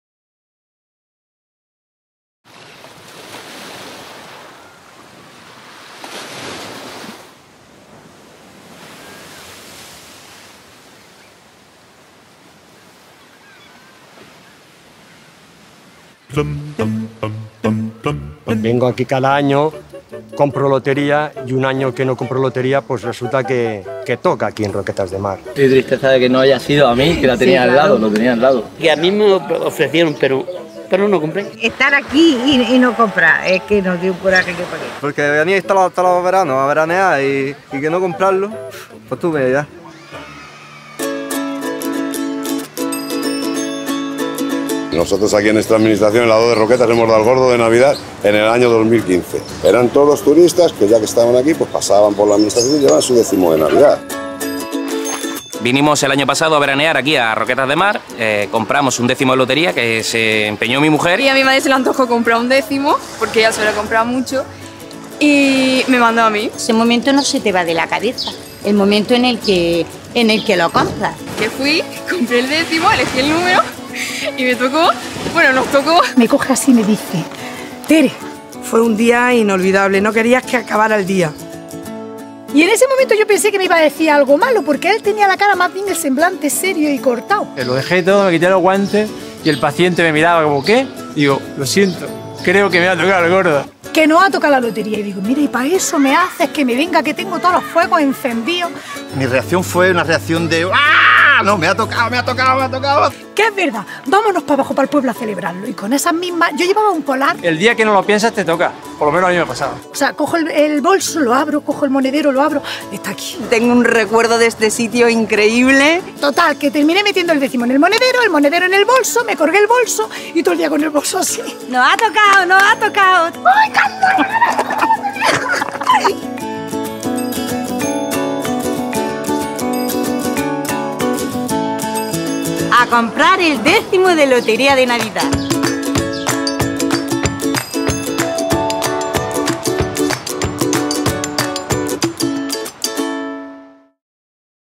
Lotería de Navidad, también en verano
Bajo el lema "¿Y si cae aquí?", la campaña de verano creada por Leo Burnett para Lotería de Navidad recuerda que el Gordo empieza a tocar en vacaciones.